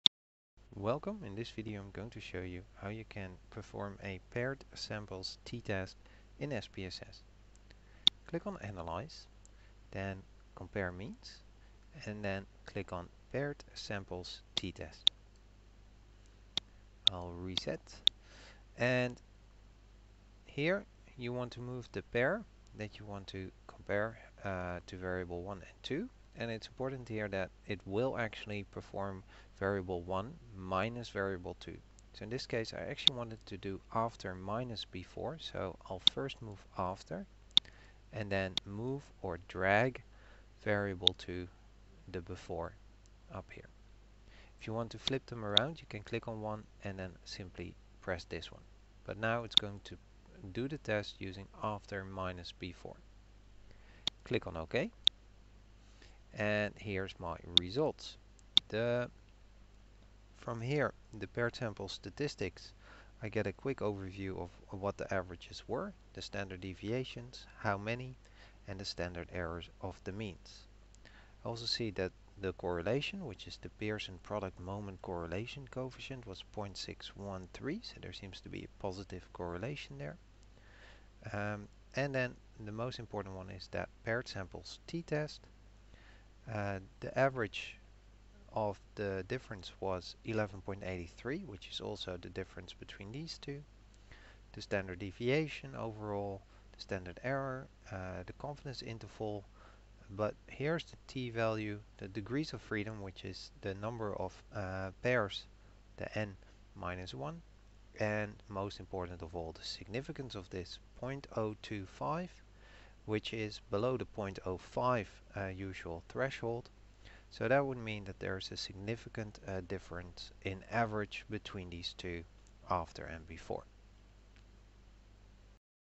SPSS - Paired samples t test
Instructional video on how to perform a paired (matched/dependent) samples t test in SPSS.